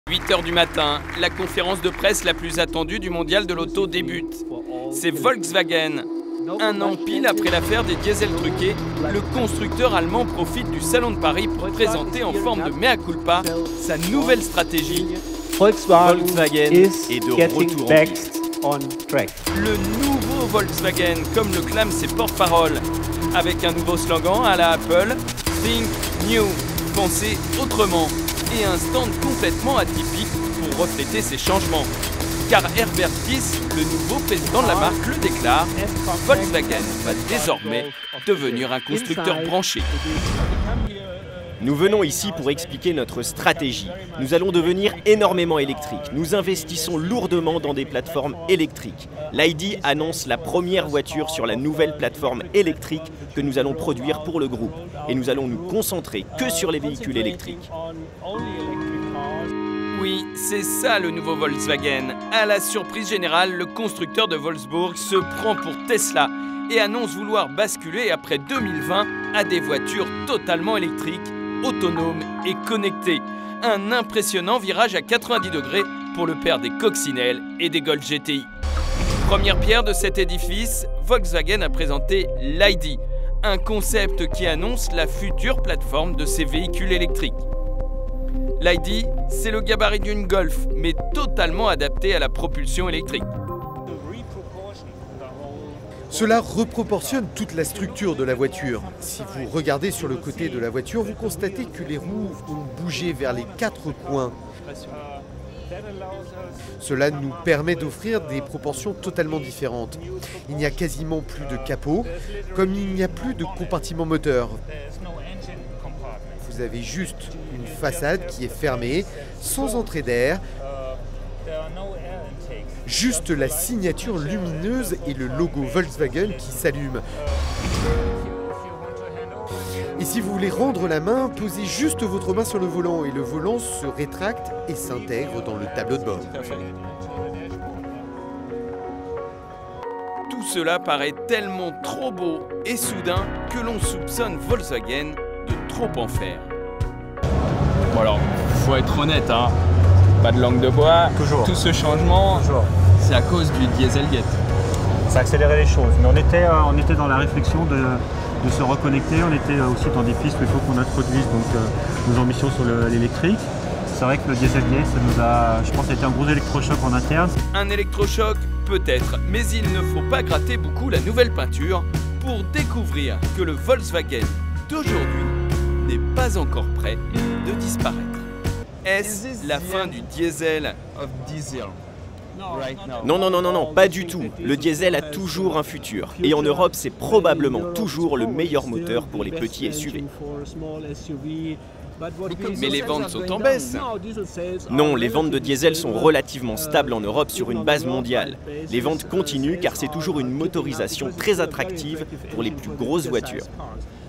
La nouvelle image de Volkswagen
Après le scandale du dieselgate, Volkswagen tente de se racheter une image